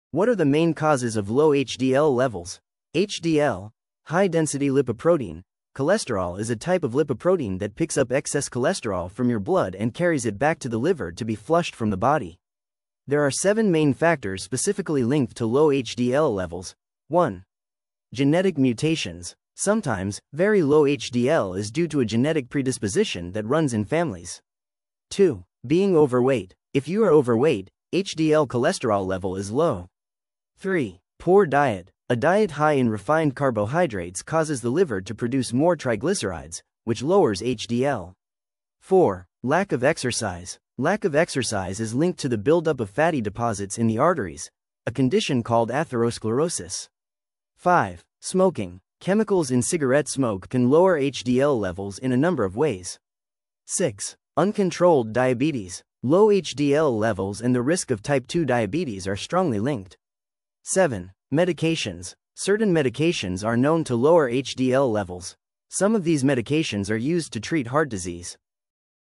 What Are The Main Causes of Low HDL Cholesterol Levels?
HDL (high-density lipoprotein) cholesterol is a type of lipoprotein that picks up excess cholesterol from your blood and carries it back to the liver to be flushed from the body.

There are seven main factors specifically linked to low HDL levels:

1. Genetic mutations
Sometimes, very low HDL is due to a genetic predisposition that runs in families.

2. Being overweight
If you are overweight, HDL cholesterol level is low.

3. Poor Diet
A diet high in refined carbohydrates causes the liver to produce more triglycerides, which lowers HDL.

4. Lack of exercise
Lack of exercise is linked to the buildup of fatty deposits in the arteries, a condition called atherosclerosis.

5. Smoking
Chemicals in cigarette smoke can lower HDL levels in a number of ways.

6. Uncontrolled diabetes
Low HDL levels and the risk of type 2 diabetes are strongly linked.

7. Medications
Certain medications are known to lower HDL levels. Some of these medications are used to treat heart disease.